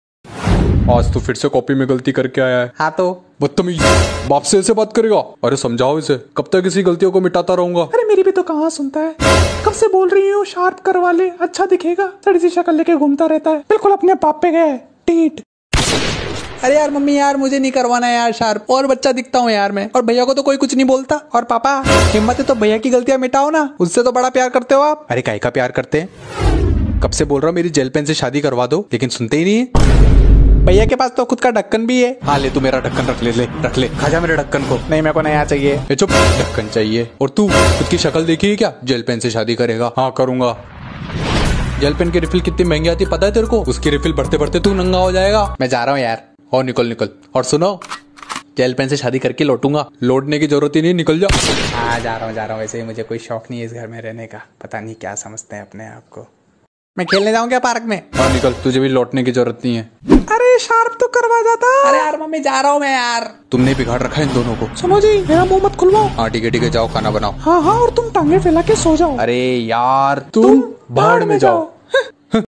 Funny Conversation of Rubber and Pencil😀😀😀
How pencil, rubber talks.. This video is all about feeling of rubber pencil and pen. Watch it and enjoy it